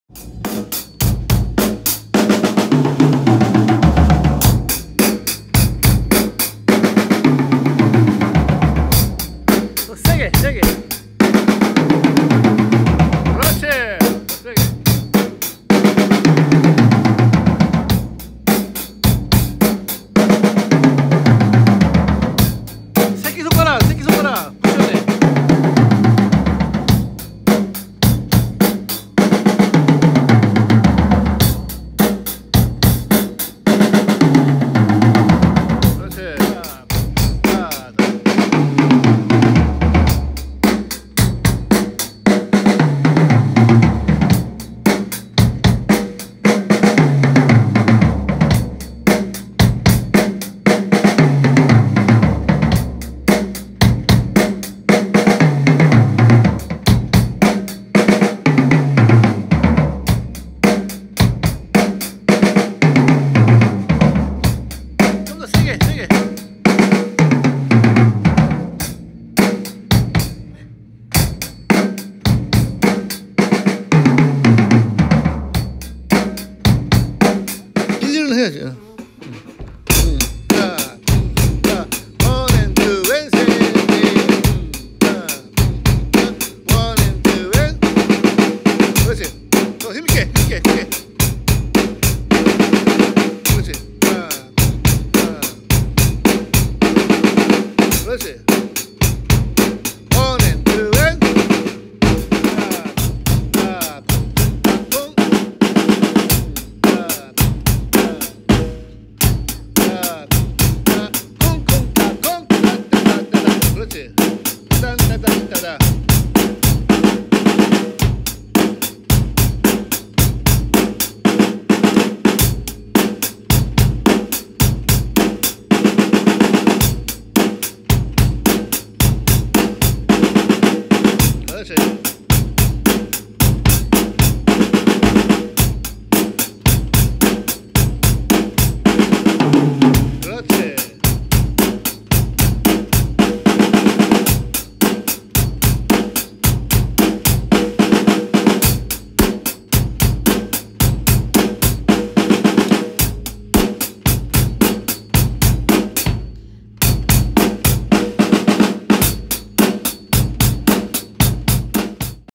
드럼레슨 엿보기01 -클레식 타악연주자가 셋트 드럼을 배우면??
권낙주-옥토드럼레슨

클레식 타악 연주자가 드럼셋트를 배우기 시작하다~~~!!
클레식에서 배운 기본기가 있어서 금방 따라하기 시작하네요~~

영상의 내용은 셋트에서 기본 리듬을 치면서 한박자필인 리듬을
연결시키는 연습입니다~

양손순서
RLRL
R-RL
RLR-
RL-L